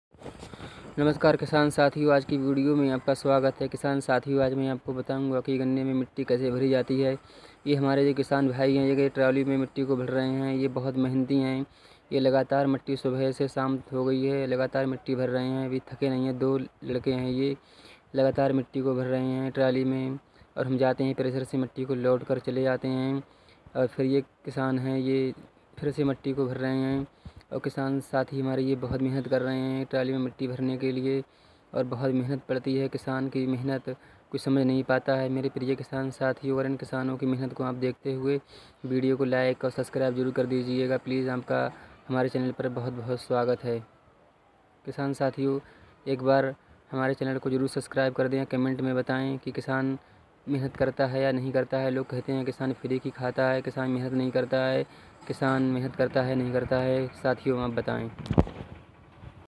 Trali mai mitti bharne ka tarika. ट्रॉली मैं लेवर मिट्टी कैसे भरती है। Hardworkkisan Indiankisan.
Trali mai mitti bharne ka tarika. ट्रॉली मैं लेवर मिट्टी कैसे भरती है। Trali Mai lever mitti bharti
cane farmer